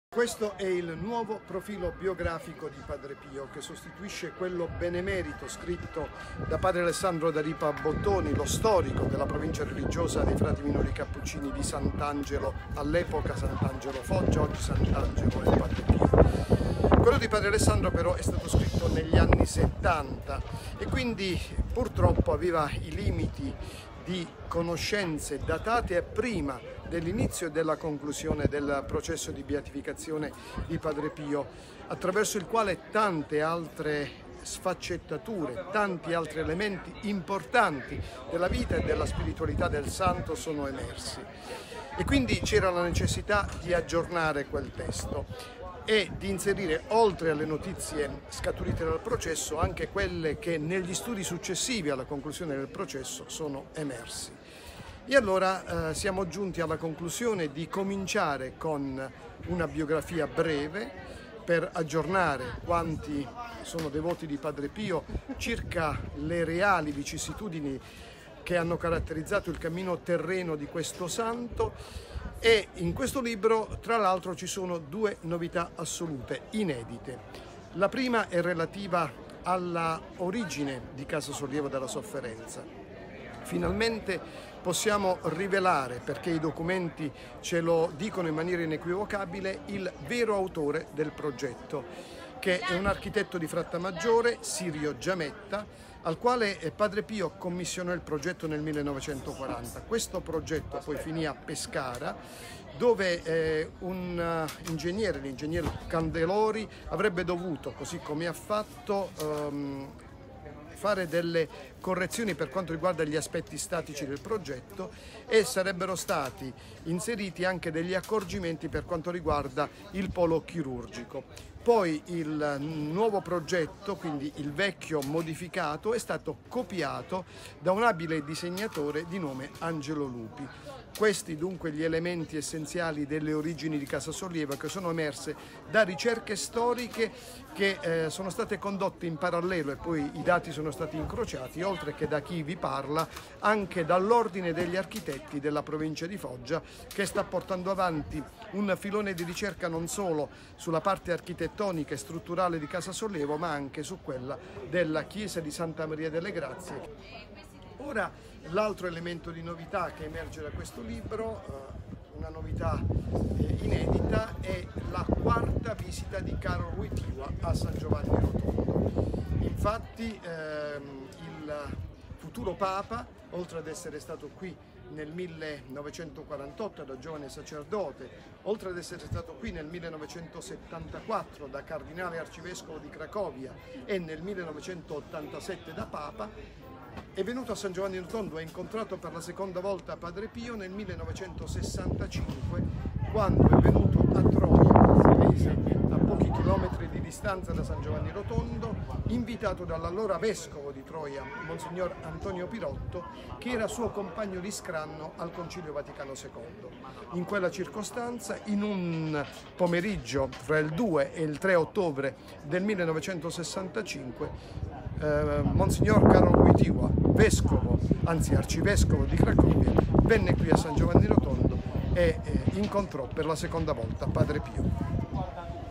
Ecco la nuova biografia ufficiale di Padre Pio da Pietrelcina
Padre Pio il cammino di un santo.
Due grandi novità nella nuova biografia ufficiale di San Pio da Pietrelcina di Stefano Campanella